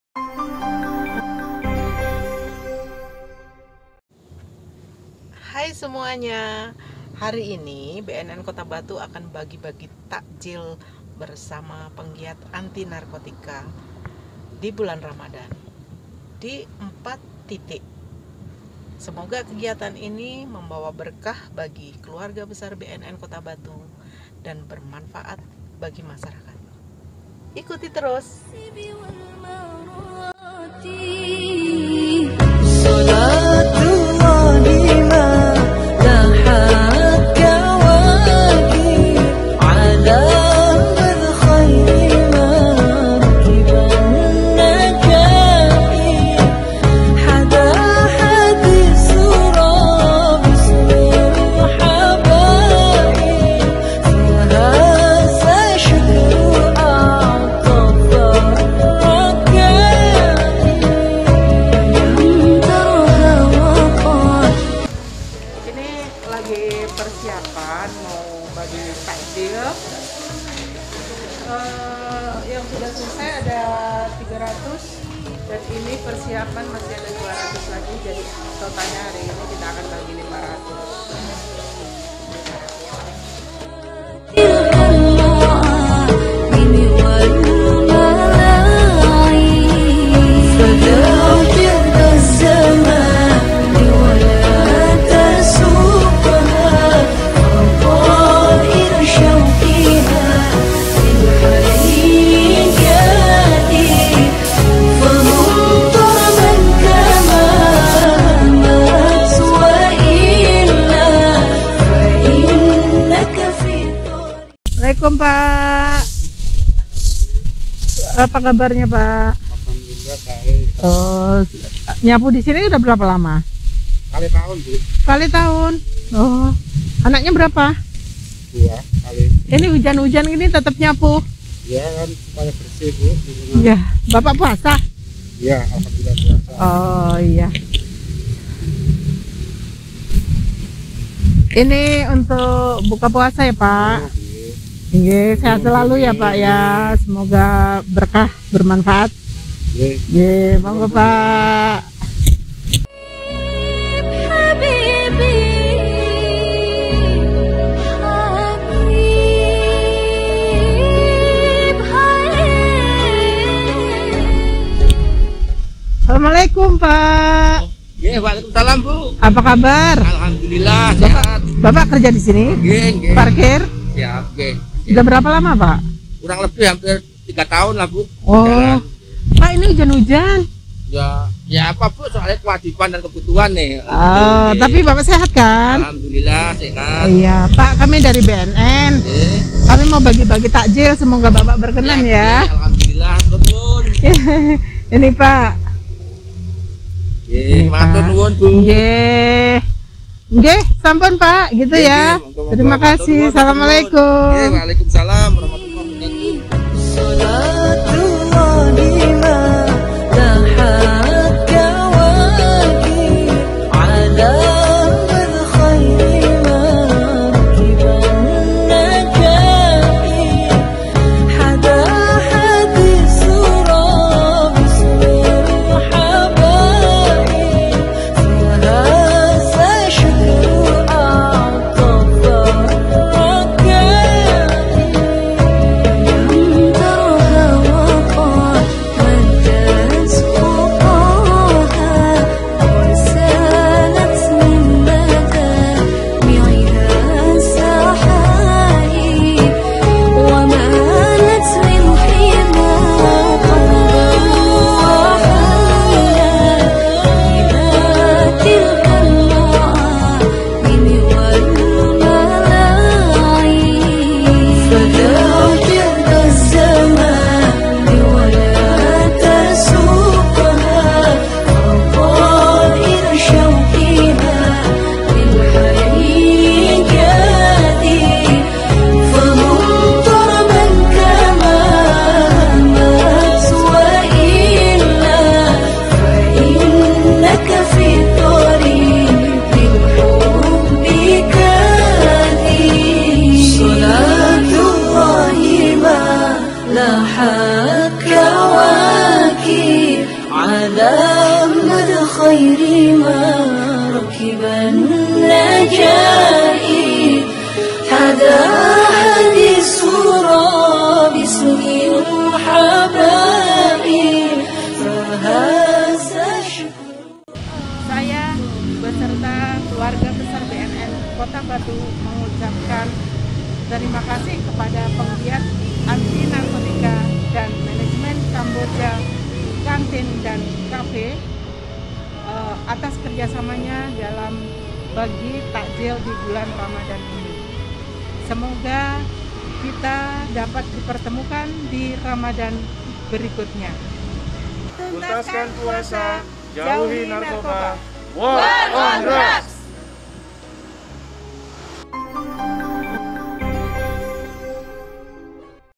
BNN Kota Batu berbagi takjil di bulan Ramadan bersama Penggiat P4GN
Masih dalam rangkaian kegiatan HUT BNN RI ke-20, BNN Kota Batu berbagi takjil bersama Penggiat P4GN di bulan Ramadan 1443 H.

Pembagian takjil ini dilaksanakan di 4 titik wilayah Kota Batu.